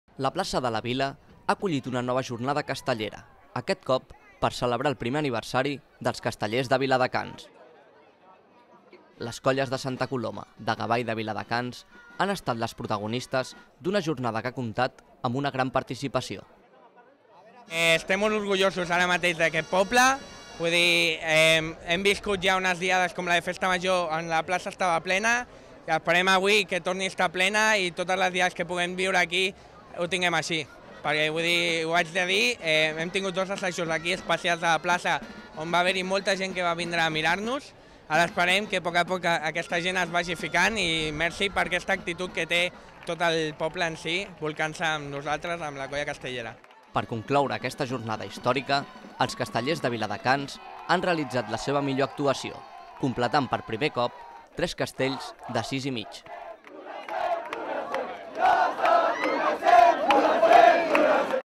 Els castellers de Viladecans celebren el seu primer aniversari amb una jornada històrica
La plaça de la vila ha acollit una nova jornada castellera aquest cop per celebrar el primer aniversari dels castellers de Viladecans. Les colles de Santa Coloma de Gavà i de Viladecans han estat les participants d’una jornada que ha comptat amb una gran participació. Per completar aquesta jornada històrica els castellers de Viladecans han realitzat la seva millor actuació completant per primer cop tres castells de 6 i mig.